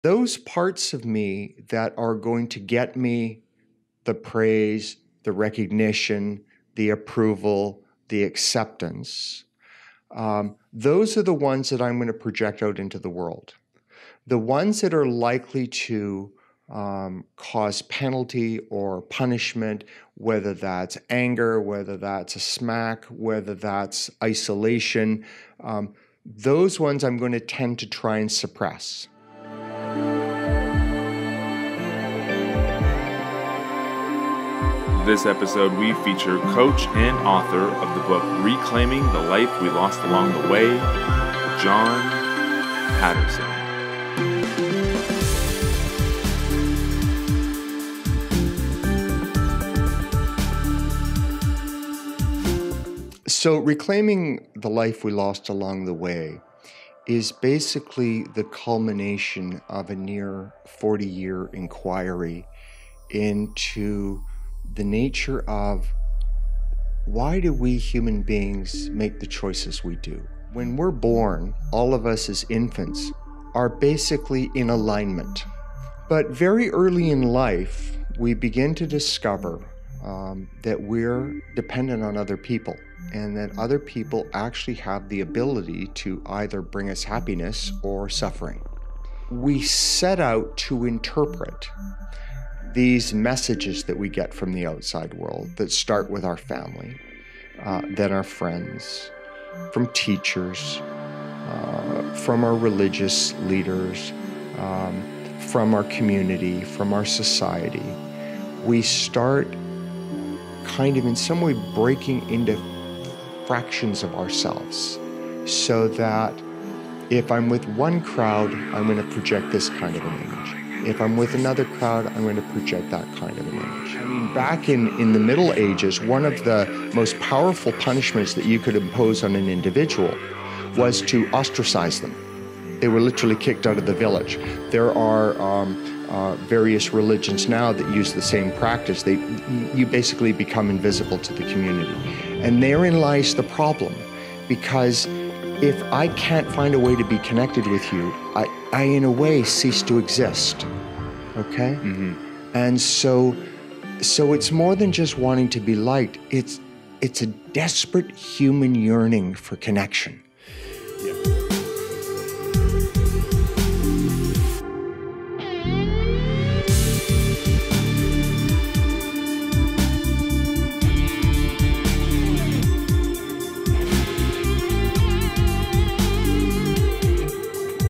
#086: John Paterson | Heart Movements - Trailer
John Paterson is an author, musician, and holds a Master's degree in Leadership. He conducts a workshop called "Reclaim your life". John is dedicated to living fully with unconditional love and simply abides by this way of life. It is his spiritual path. Despite overcoming alcoholism at an early age, John now has a life-long passion to discover how human beings can access and express their deepest and broadest potential.